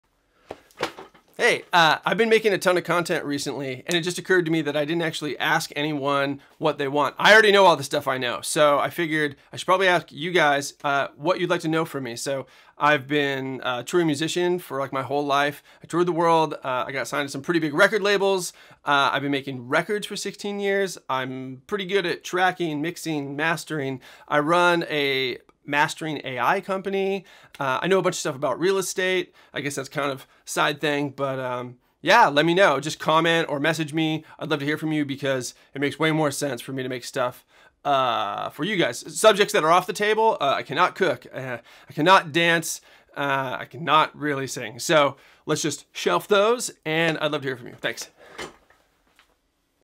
What do YOU want?
Can you guys help me by telling me the kind of content that would be helpful for you? Comment or message thank you!